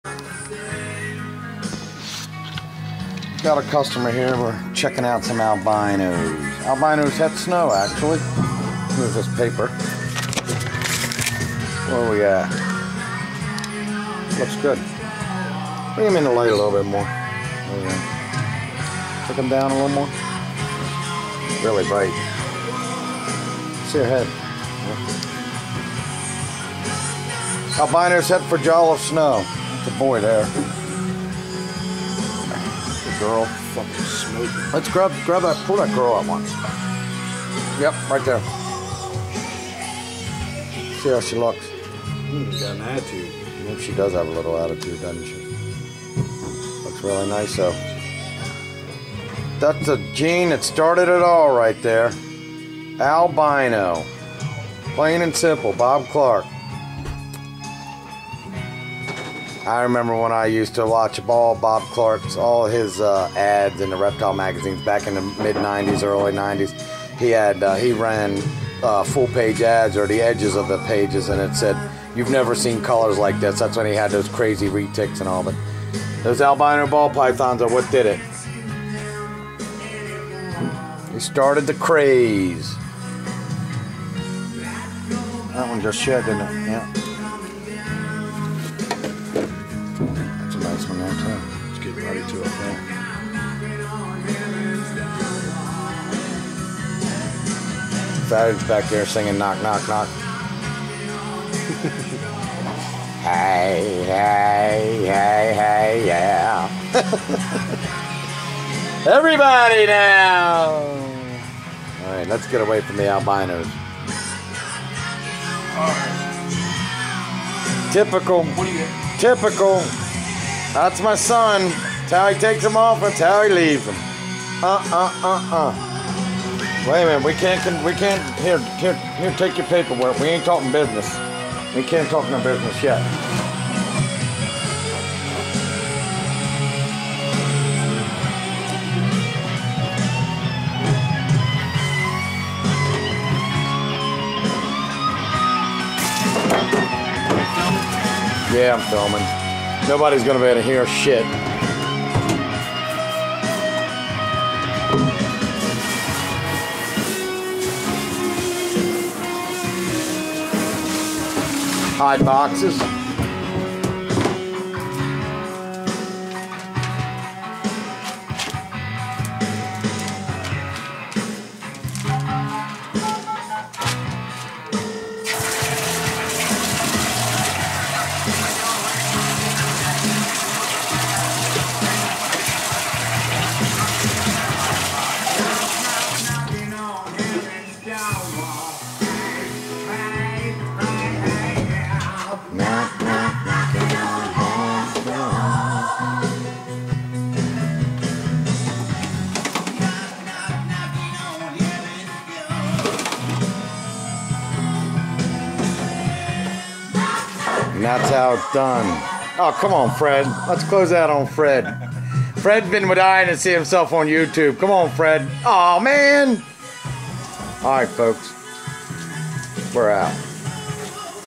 Knock, Knock, Knockin'.........
Vid Clip of some 07 Albinos het Jolliff Snow and some bits on how we doooooozzzzzzzz it!!. Who's Got The Balls?". Rock On!!